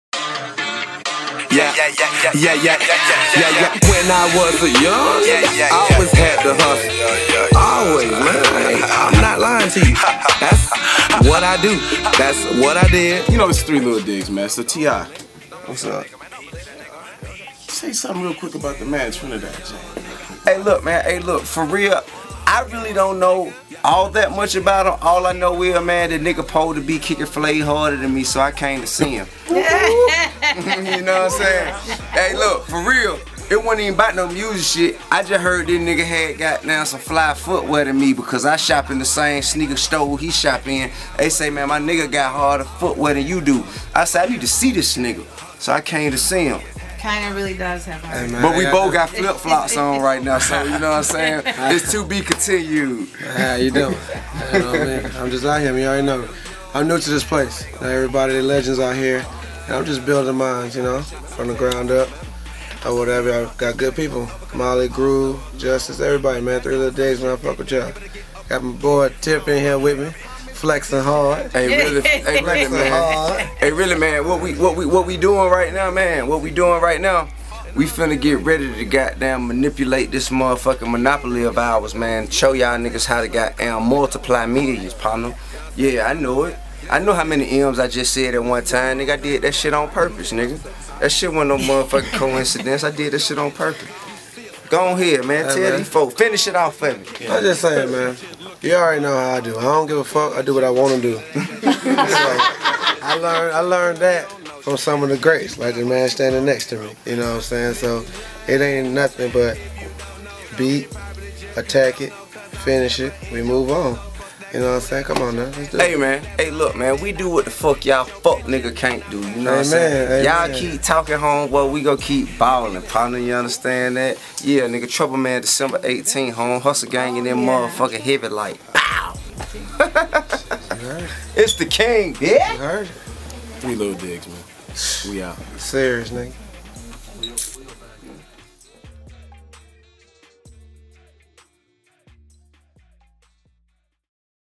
Studio LIfe: T.I. and Trinidad James in the lab
Watch as T.I. stops by 3 Little Digs Headquarters to check in on the homie Trinidad James.